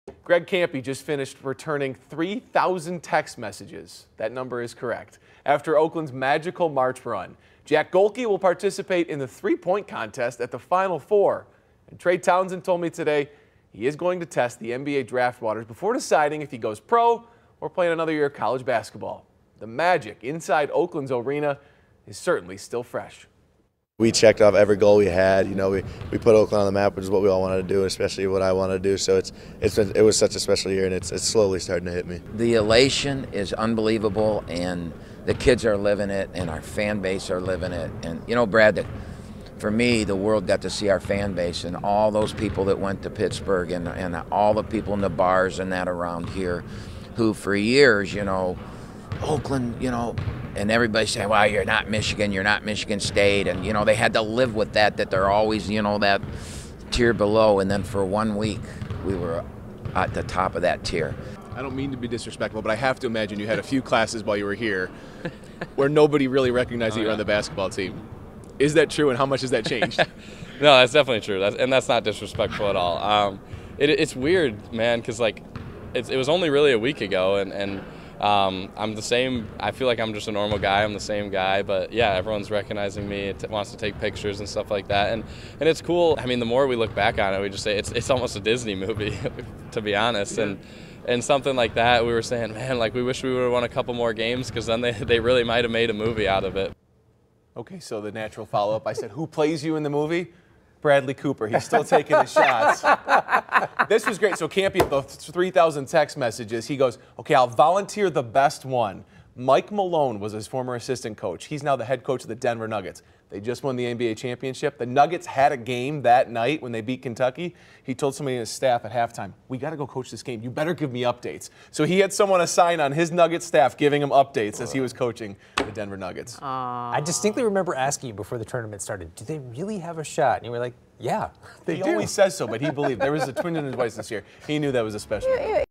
Oakland's Greg Kampe, Jack Gohlke, Trey Townsend talk with Brad Galli
Oakland's Greg Kampe, Jack Gohlke, Trey Townsend talk with Brad Galli about the magical March run